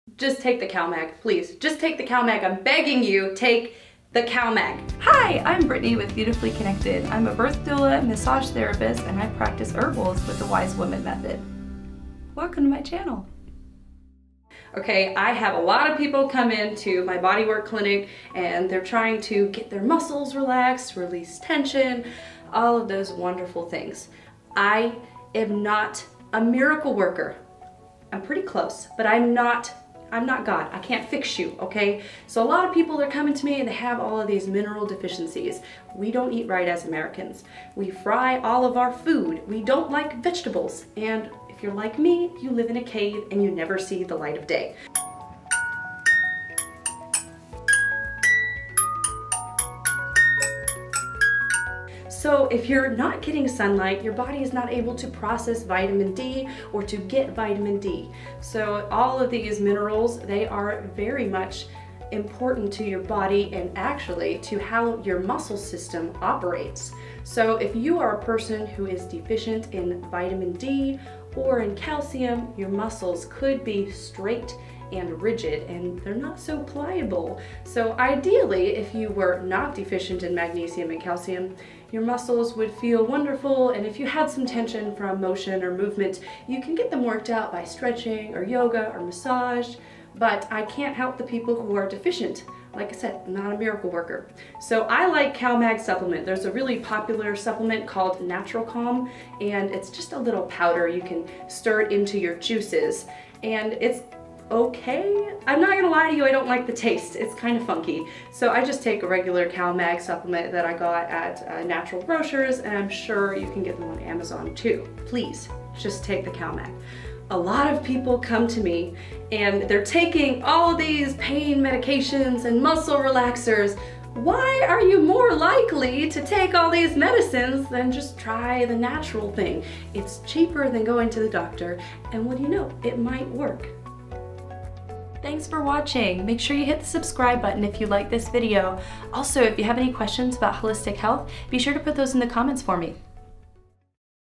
Avoid Muscles Soreness with this Simple Trick | Just Take The Cal-Mag
Flexeril, Oxy, Lortab...Do you take these for muscles pain?
Have you ever considered a simple, natural, legal solution?

Big pharma like customers, but I like to keep people well. JUST TAKE THE CAL-MAG

Have questions about holistic health and wellness? Leave them in the comments and we will get reply back and make another video to help you out.